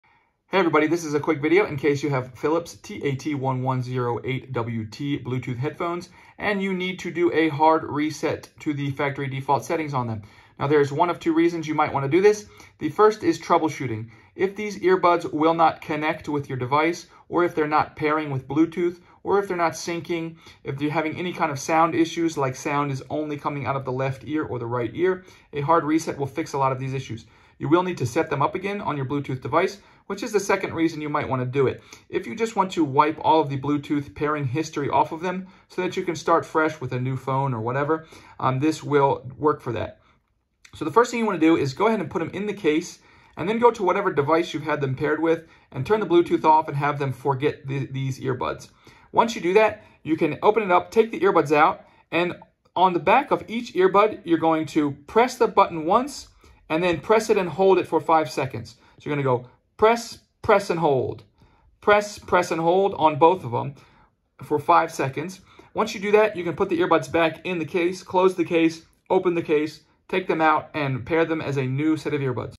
Hard Reset Philips TAT1108wt Earbuds
How to hard reset the bluetooth on Philips TAT1108wt Earbud earphones if they are not connecting or not pairing or there is only sound out of the left ear or the right ear or you need to troubleshoot other problems, a hard reset to factory default settings can fix a lot of issues.

If you need new earbuds, these are some of the highest rated on Amazon